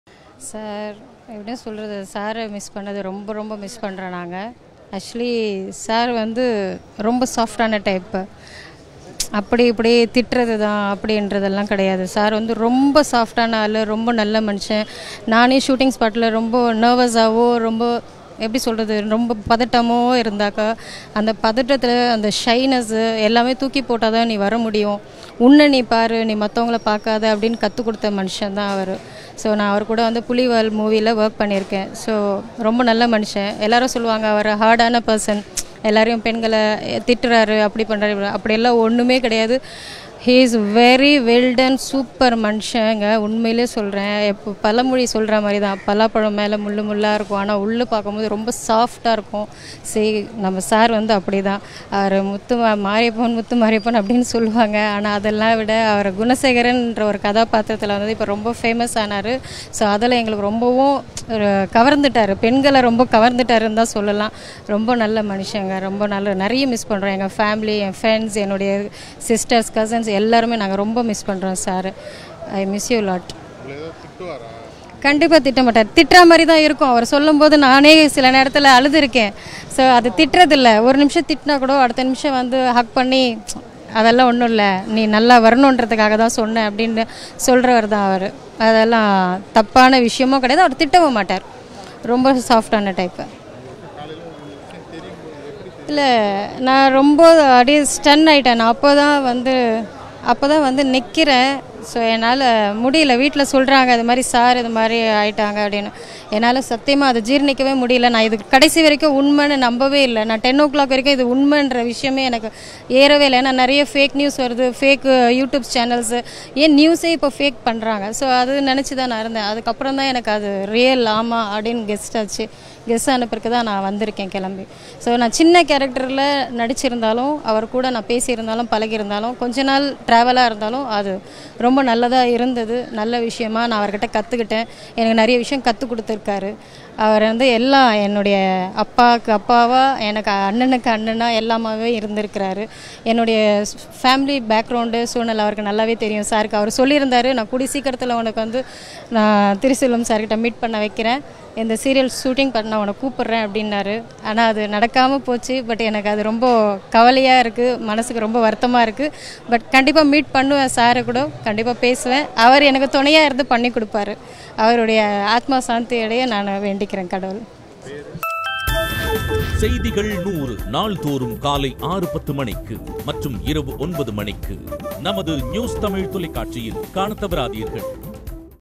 மாரிமுத்து என்ன திட்டுவாரு... பலாப்பழம் மாதிரி அவர் Character... | RIP Marimuthu | Actor Marimuthu
மாரிமுத்து என்ன திட்டுவாரு... பலாப்பழம் மாதிரி அவர் Character...